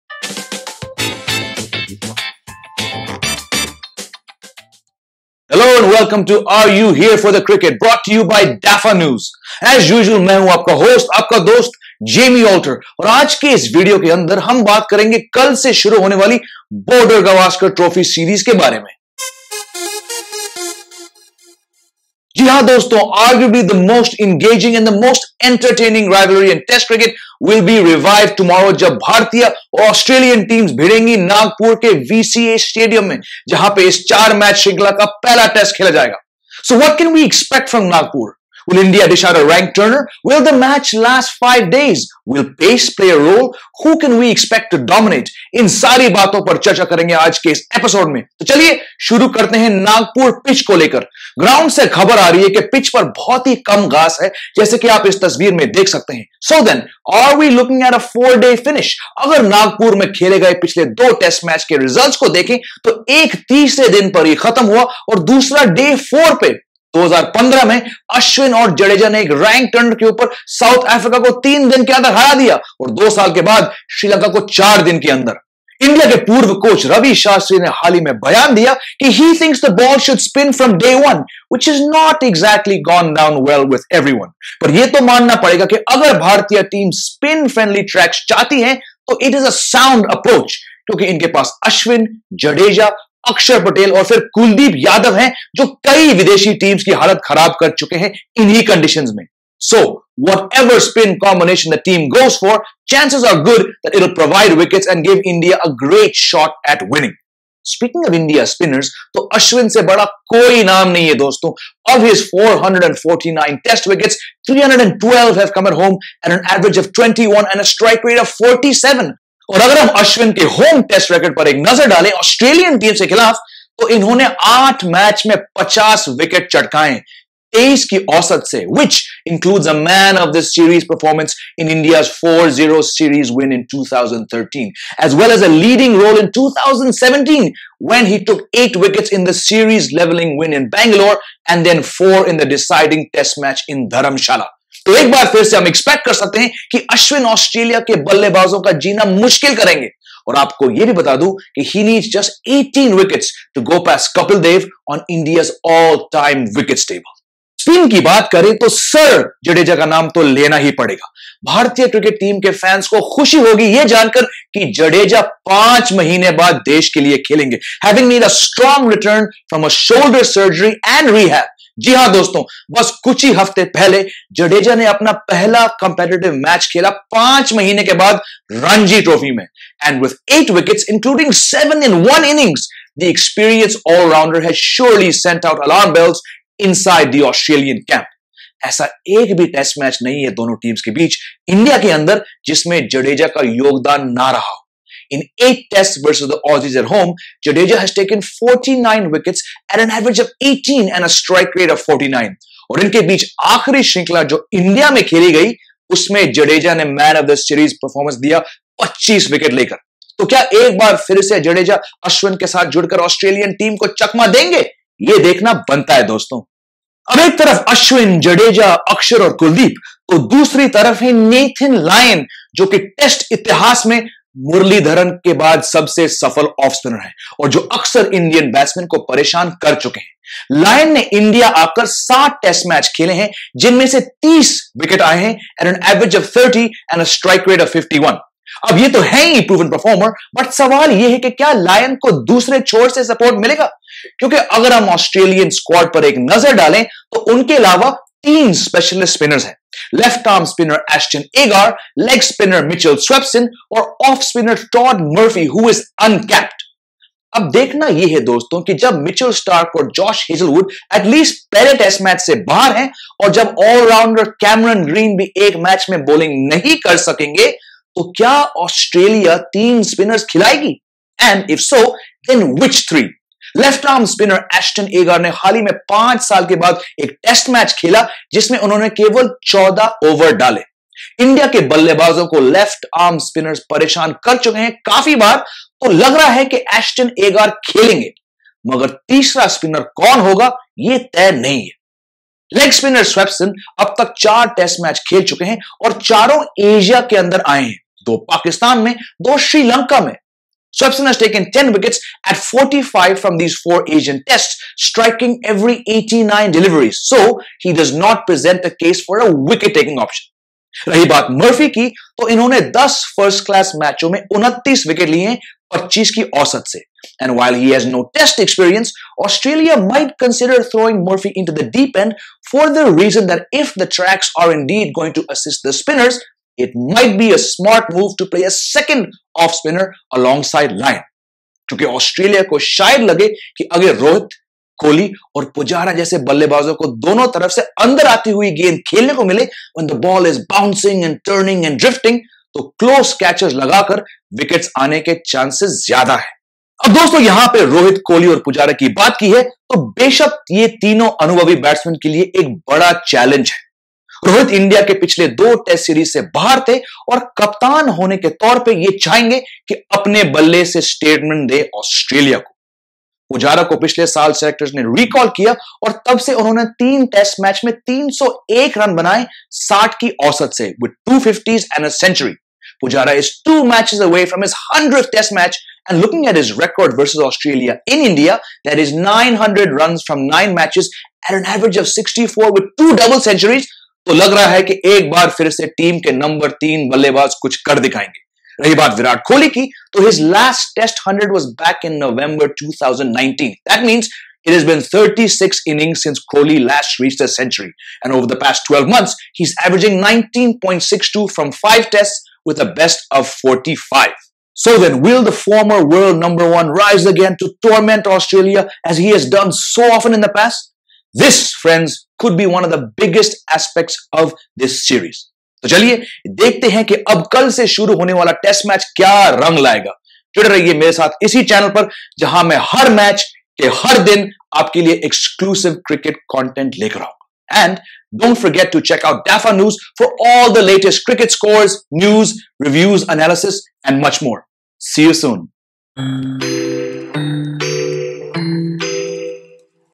BGT Test series preview | IND vs AUS | Prediction, team news, pitch report
Download the DafaNews App for the latest sports happenings around the world. Also join DafaNews Telegram group through the

The much-awaited Border Gavaskar Trophy starts in Nagpur from February 9 and in this video you will find out about some key India vs Australia stats, the key players, head to head battles and more.

In association with @dafanewsin